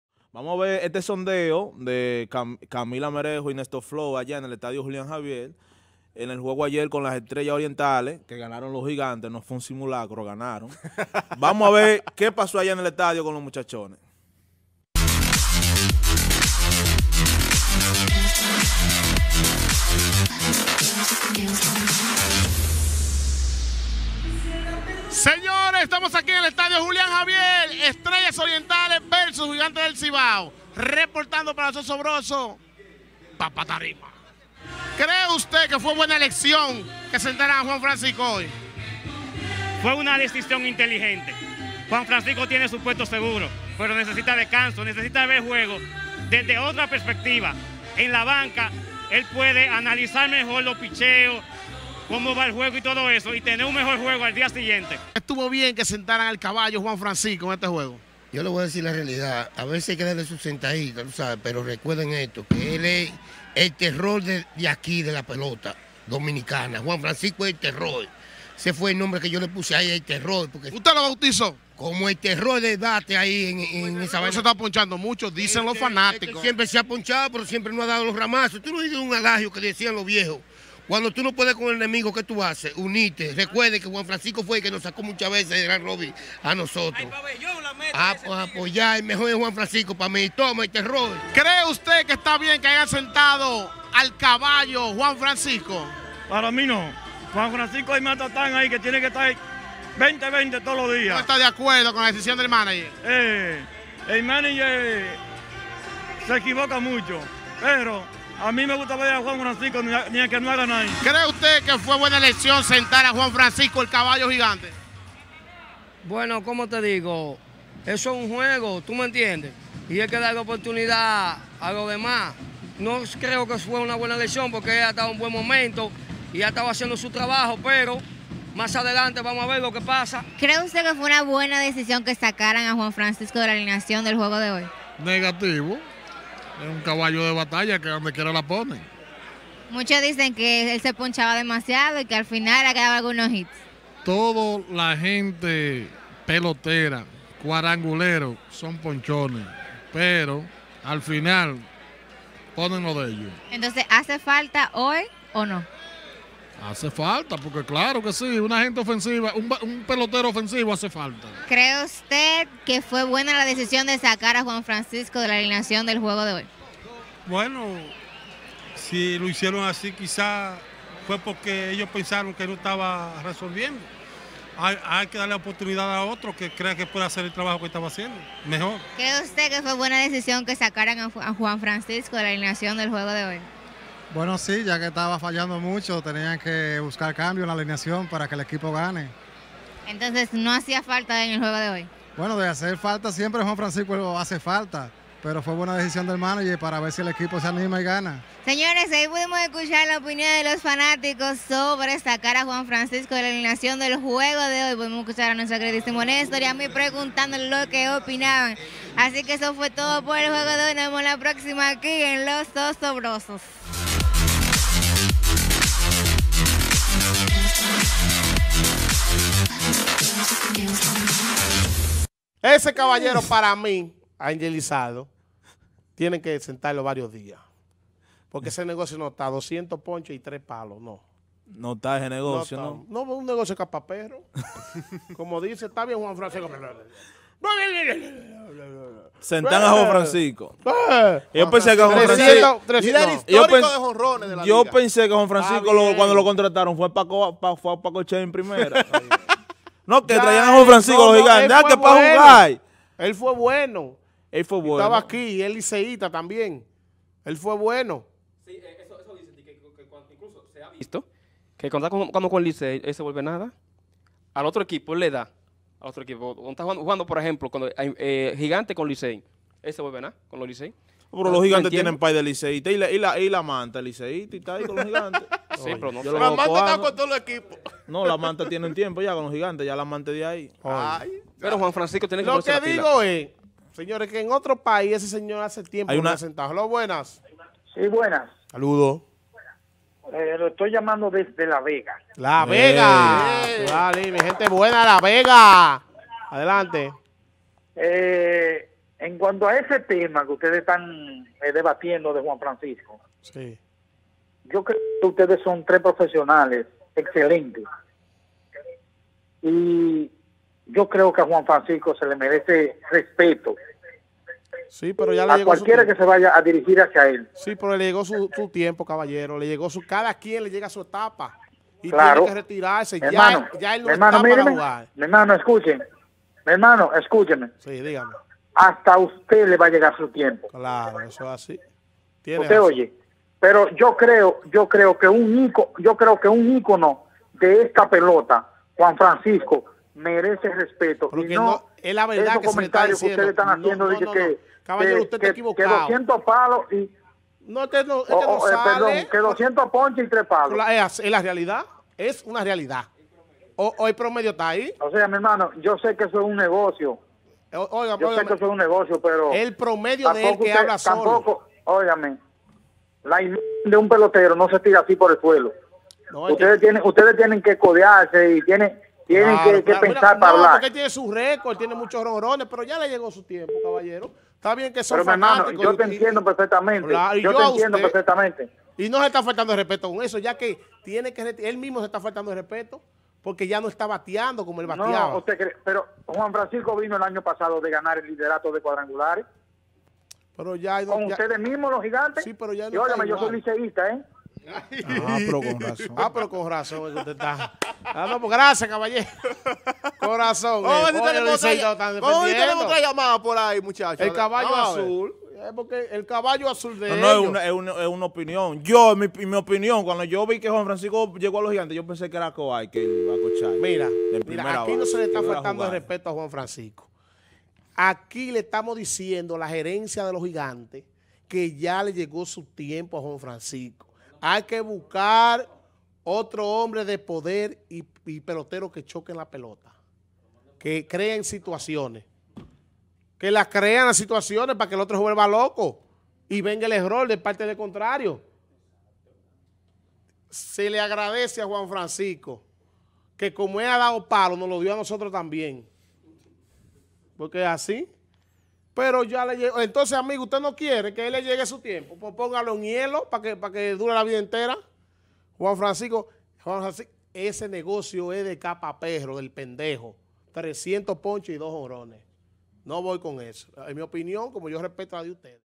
Fanáticos de los Gigantes del Cibao opinan sobre Juan Francisco en la banca
Sondeo de Los Zozobrosos desde el Estadio Julián Javier